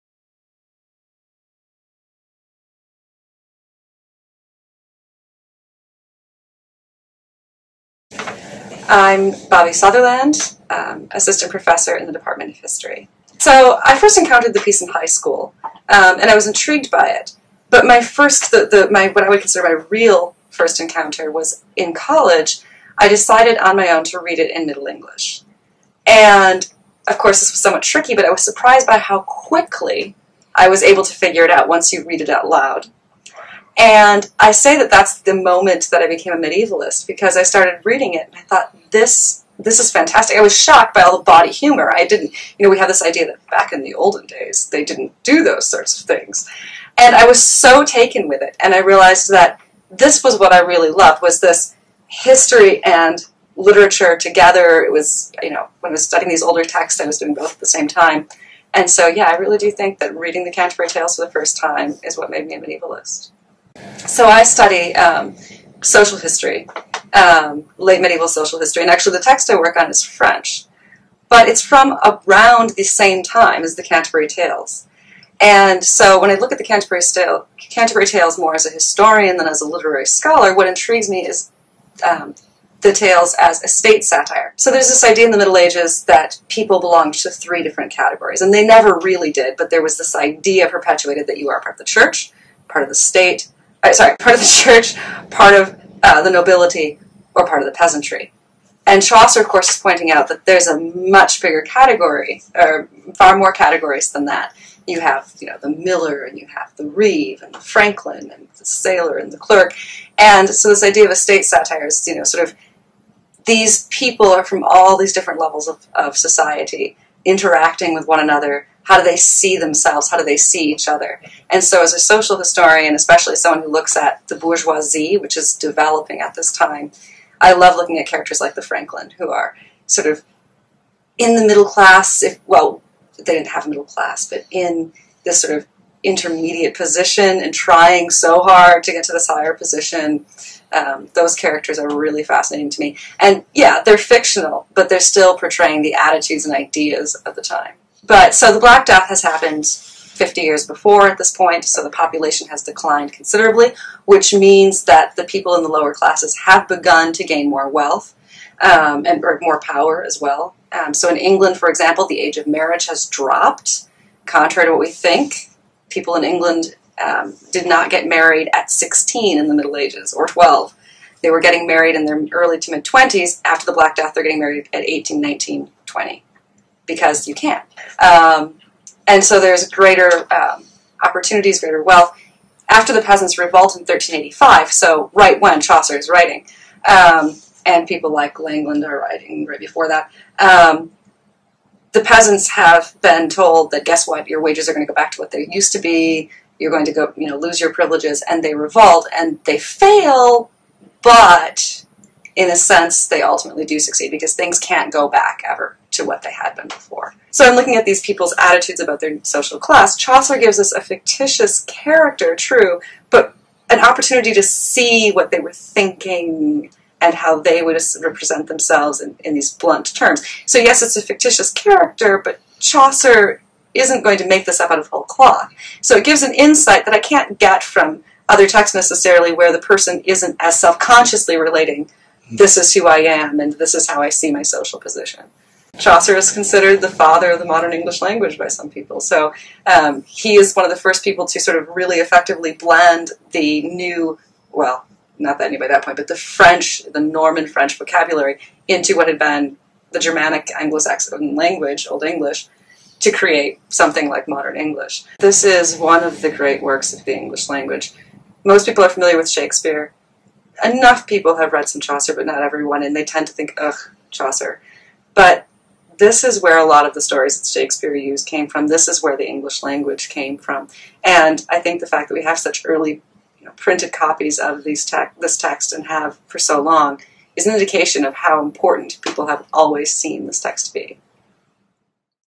Chaucer: Canterbury Tales
Bobbi Sutherland, assistant professor of English, reflects on her first experiences reading the Canterbury Tales and discusses the significance of Geoffrey Chaucer’s work for medieval historians. Interview is an online supplement to the University of Dayton exhibit Imprints and Impressions: Milestones in Human Progress—Highlights from the Rose Rare Book Collection, held Sept. 29 through Nov. 9, 2014.